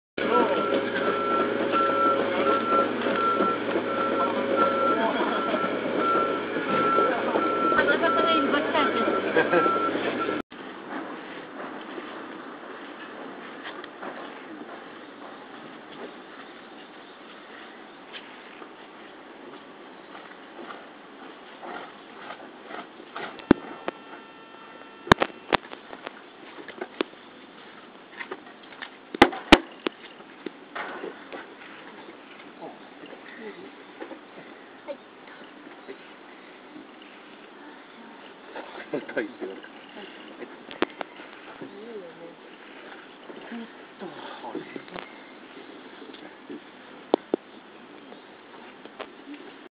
筆記体な動きをするハリガネムシ
虫嫌いな人は見ない方がいいですな。
カマキリなどに寄生する虫だそうです。私は初めて見ましたので思わず動画に撮ってみました。
工事現場で仲間が見つけました。土のう袋上を這わせてみたら，アルファベットの筆記体を書いてるようにも見えました。